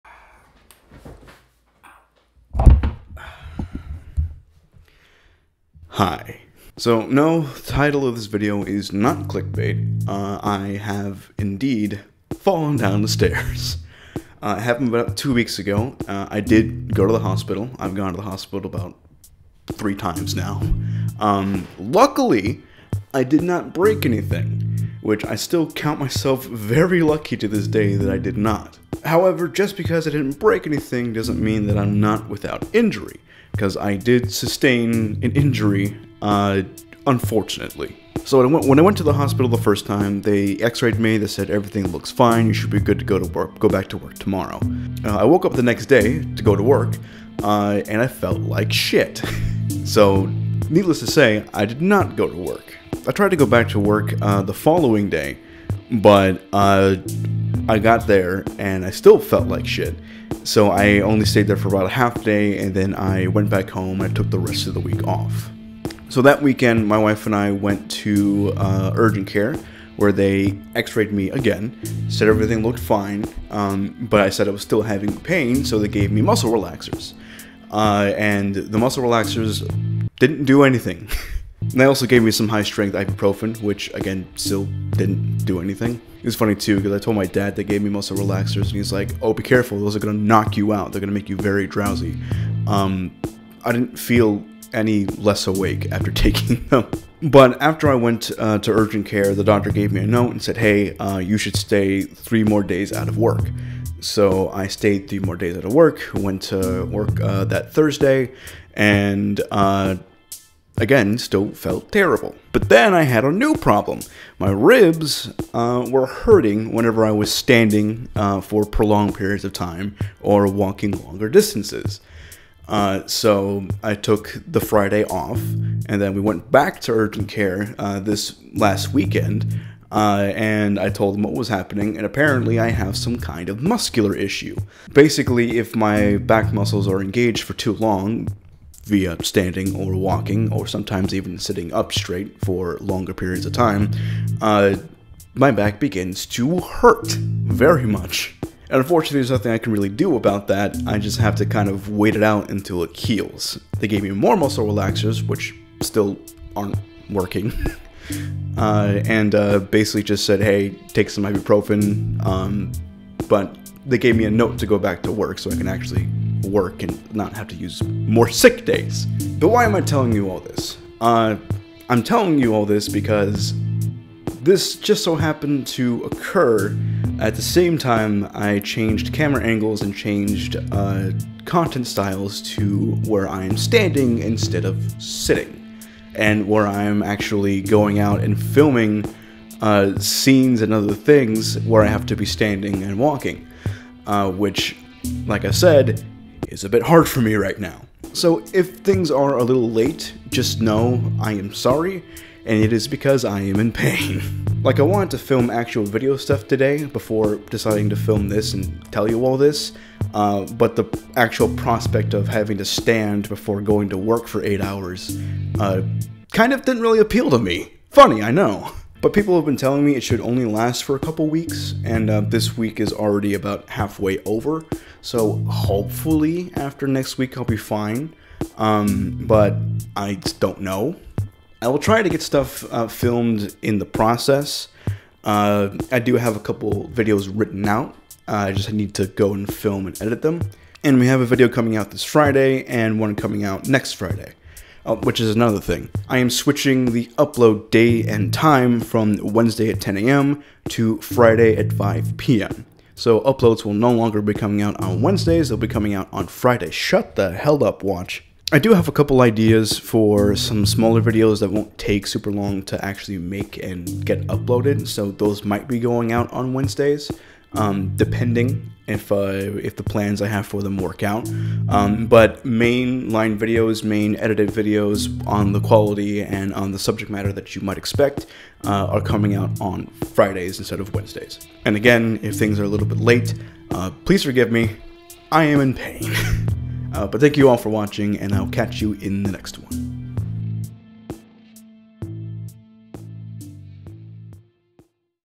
I Fell Down the Stairs (+Other Announcements)
I fell down the stairs...OH! And uploads will now be on Fridays at 5pm!